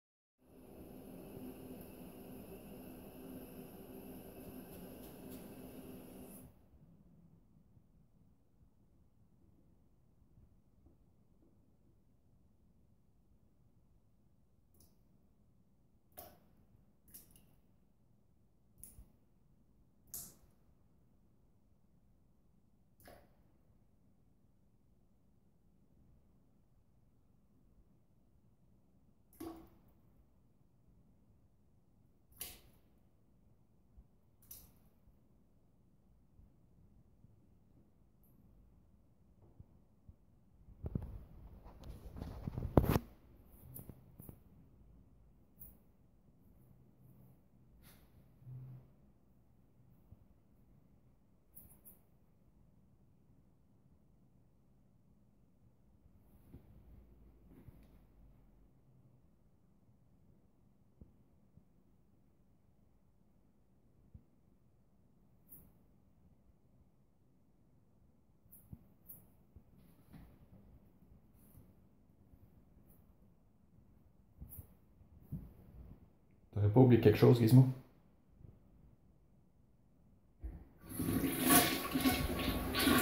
Cat using toilet and Flushes 1/2
Who said cats are stupid